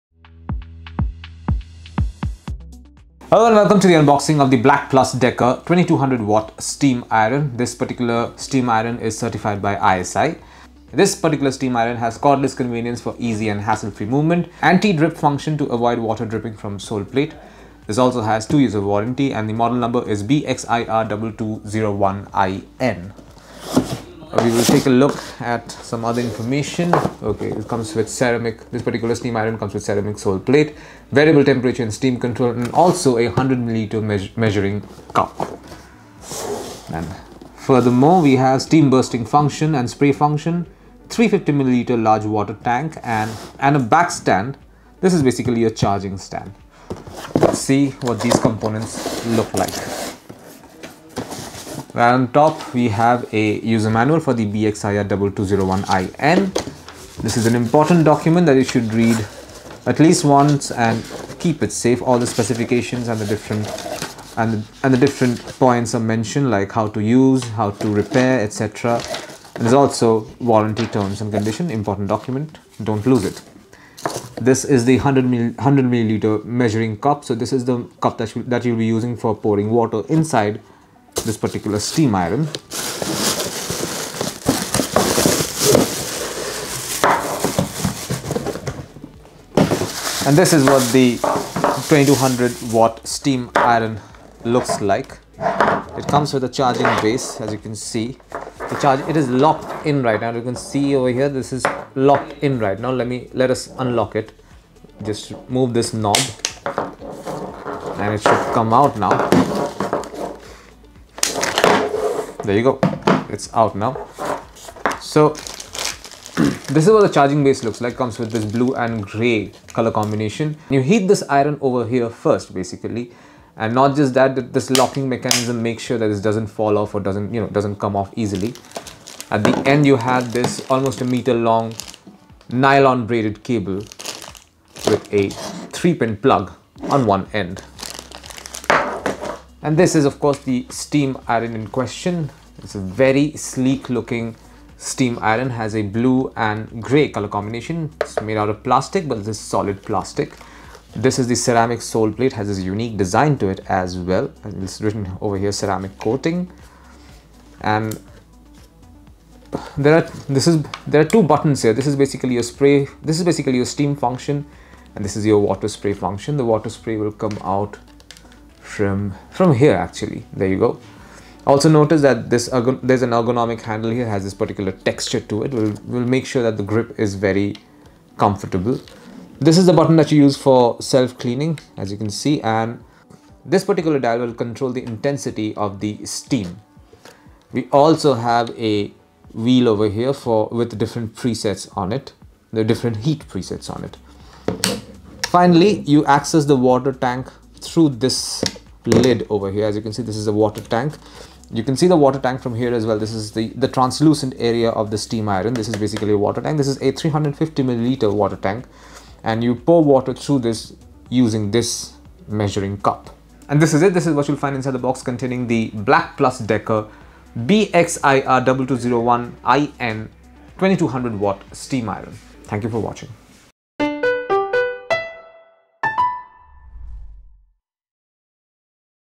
Black+Decker BXIR2201IN 2200W Steam Iron Unboxing
About the Black+Decker BXIR2201IN 2200W Steam Iron:
Cord and cordless convenience for easy and hassle free movement;Ceramic coated sole plate - even heat distribution and best for all types of clothes
30 g/min continuous steam output and 100g/min steam boost;Visible water droplets in the water tank means the iron was tested in factory and passed the quality test
Anti-drip feature to avoid any water dripping from the soleplate in case the steam level is not reached;350 ml water storage tank , Cord length :1 m
Vertical steam : Ideal for all fabrics and saves time;Steam burst function and spray function; Self clean feature to clean the scales built up;Warranty: 2 Years Warranty
Care Instructions: Handle With Care

Black+Decker BXIR2201IN 2200W Steam Iron,steam iron,best steam iron in india,best steam irons 2022